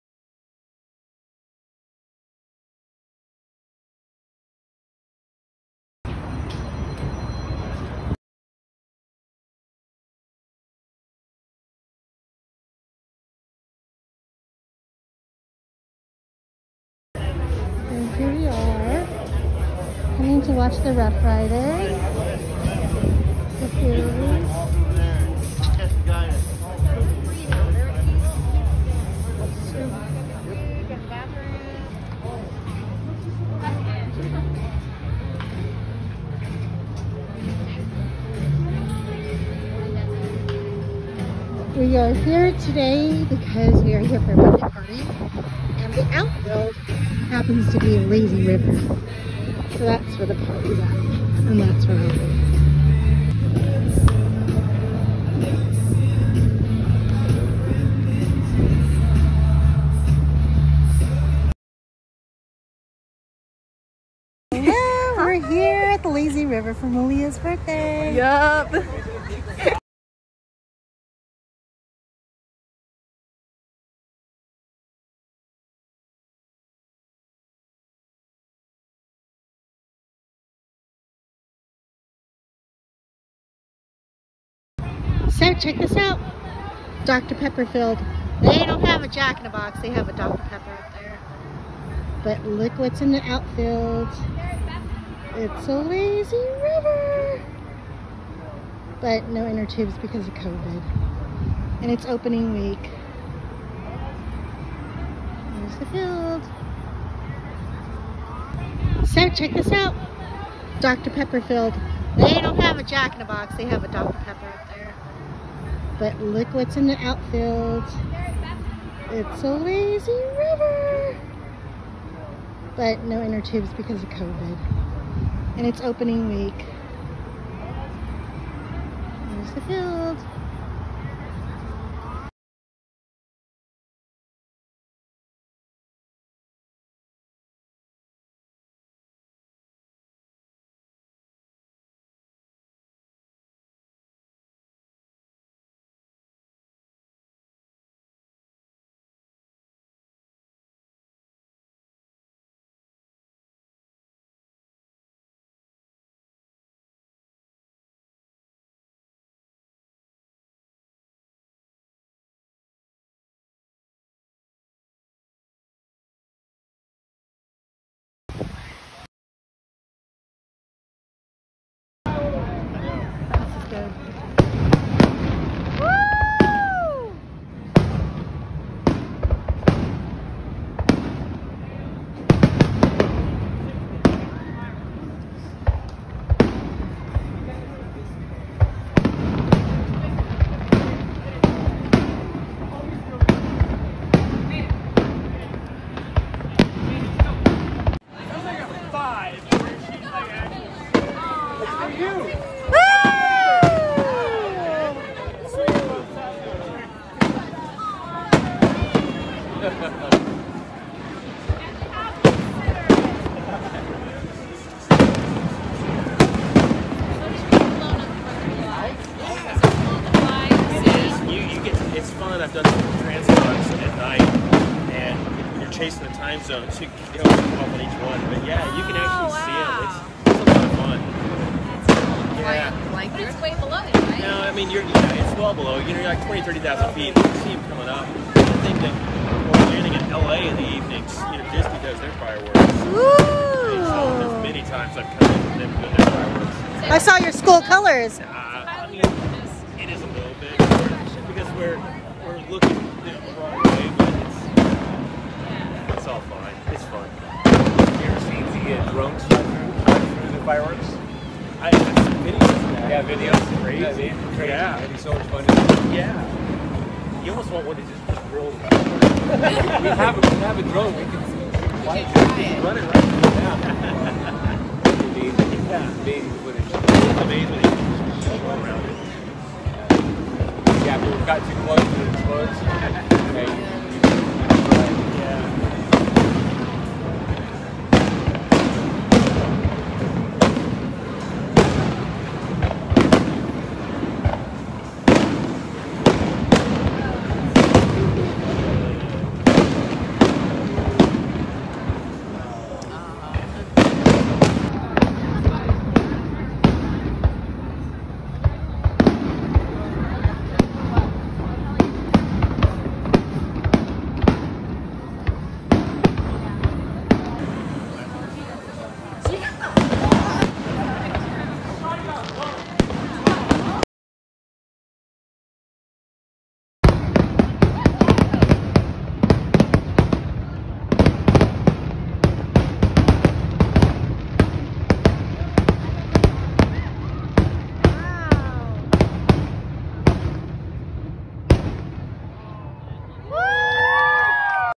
Visiting Dr Pepper Ballpark MILB
On May 7,2021 I Went to Frisco, Texas for Malia’s sweet 16 at the lazy river which lays behind the outfield of the Rough Riders baseball field.